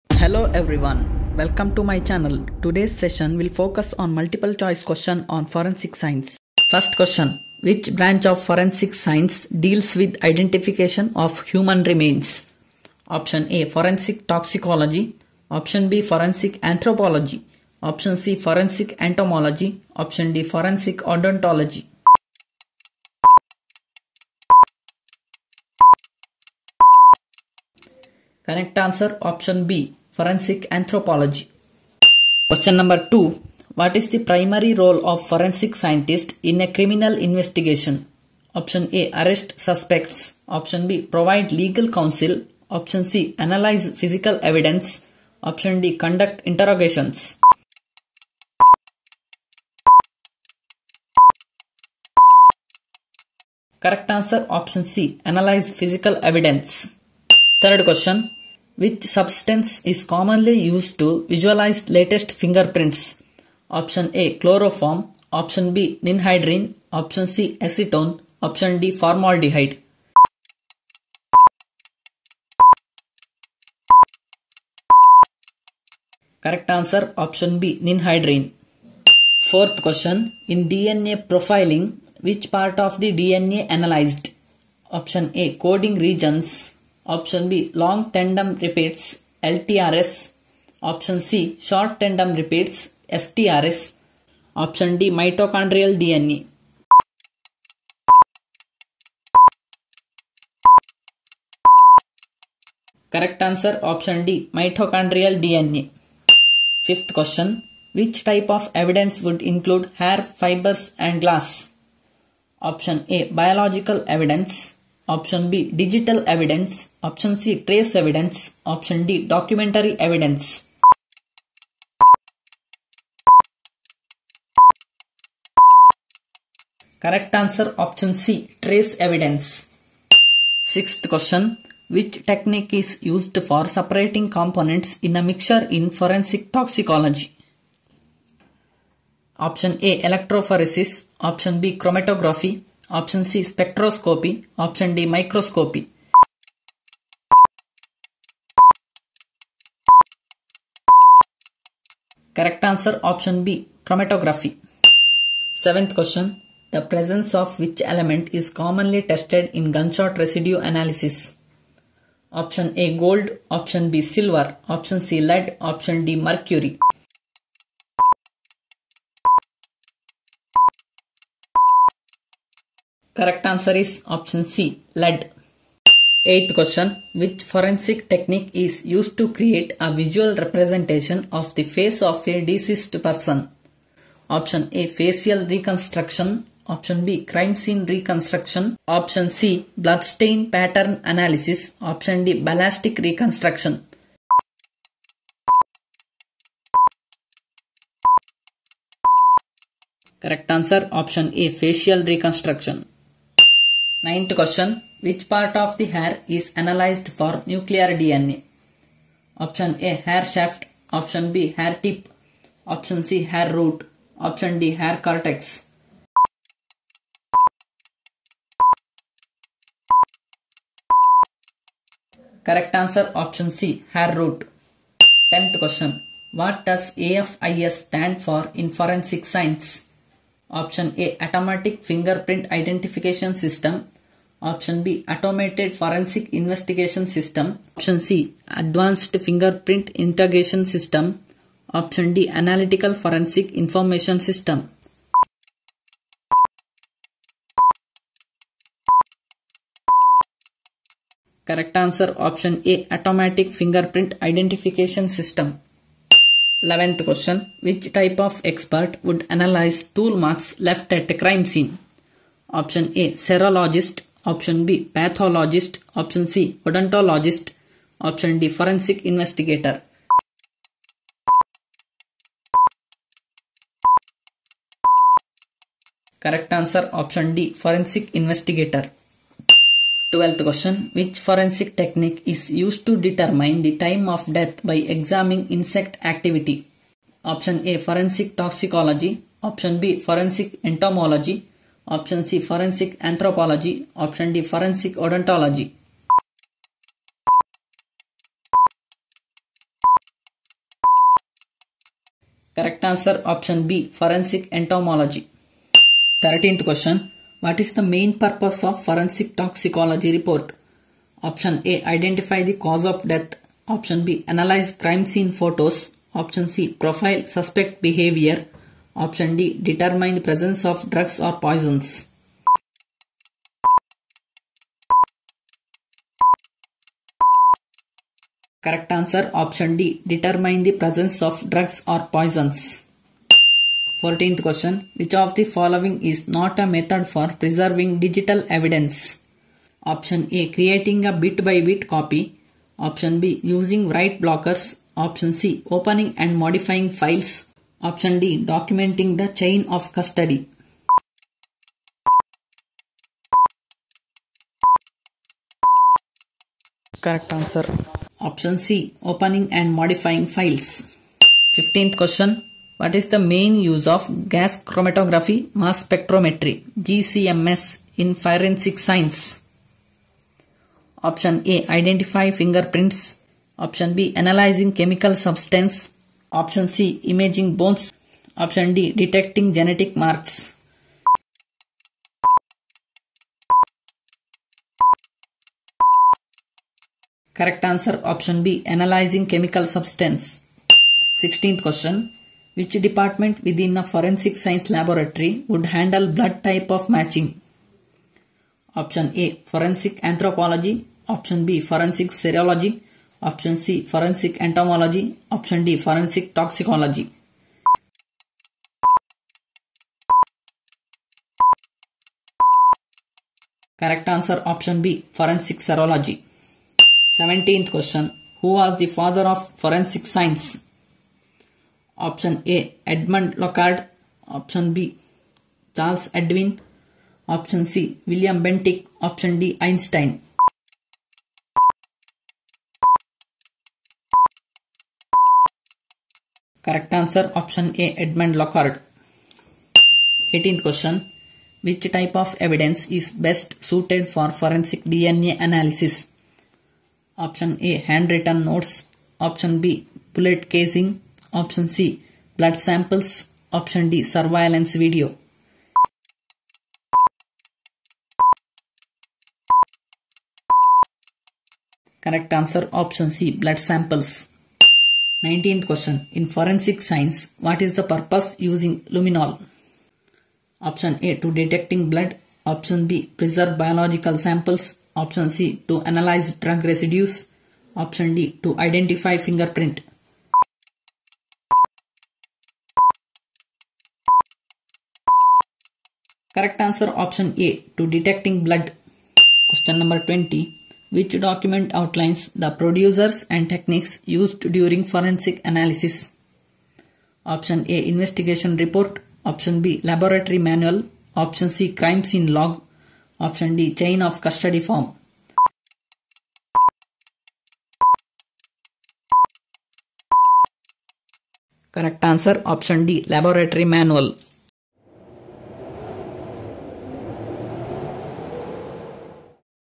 Basic Forensic Science mcq with answers | general knowladge questions for competitive exams
Hello guys,
Welcome back, In today  video we are going to learn some of the most important general knowledge questions and answers that will keep you ahead of anyone else. hope you guys are enjoying this gk collections.

Your Quaries:
1. general forensic science mcq
2. forensic science for UGC NET UPSC